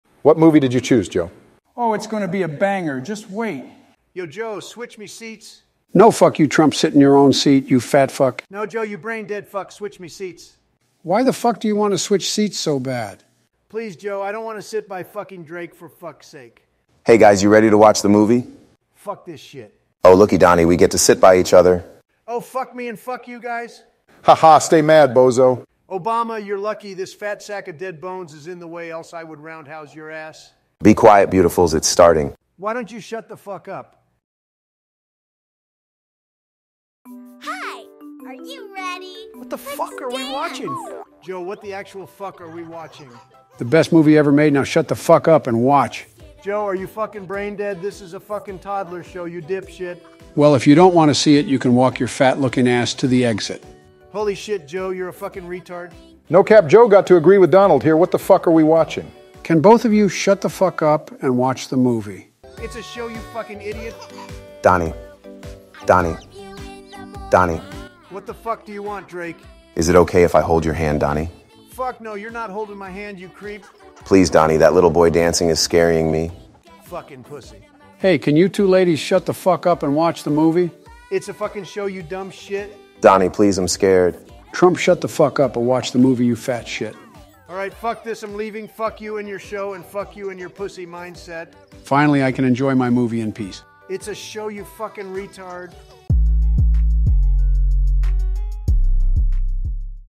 U.S President watch a movie with drake
U.S president go to the movies and invite drake. Trump was not happy